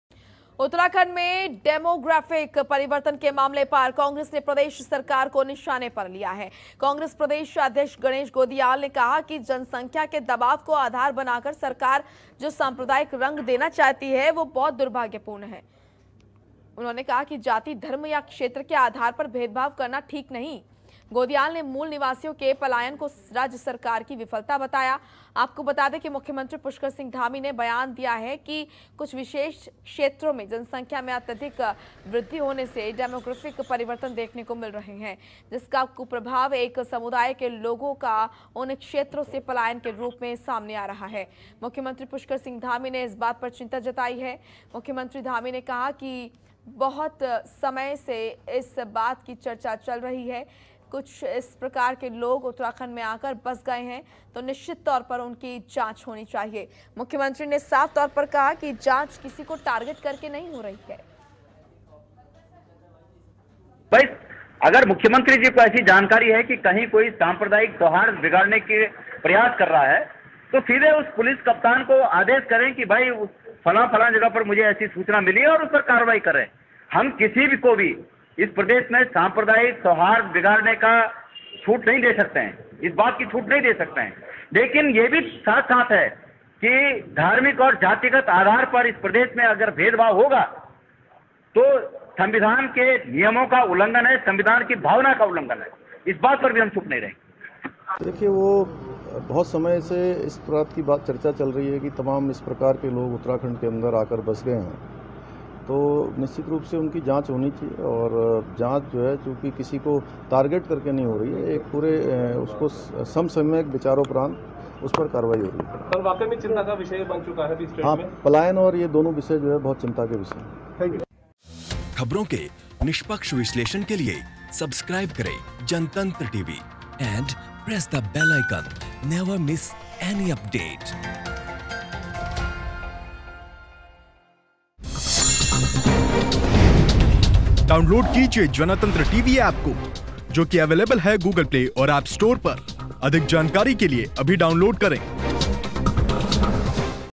Uttarakhand Demographic Changes | Congress Targeted the state Govt | Ganesh Godiyal
About Video

डेमोग्राफिक परिवर्तन पर सियायत तेज
कांग्रेस ने प्रदेश सरकार को निशाने पर लिया
साम्प्रदायिक रंग देना चाहती है सरकार- गणेश गोदियाल

In This Video:
Uttarakhand Demographic Changes
Congress Targeted the state Govt
Ganesh Godiyal
uttarakhand News today
cm pushkar dhami
congress ganesh godiyal
uttarakhand congress